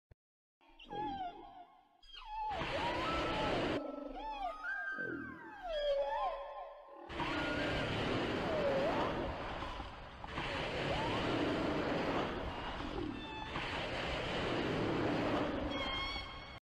✅Ballena jorobada en el puerto💥
Parece ser que esta ballena era muy curiosa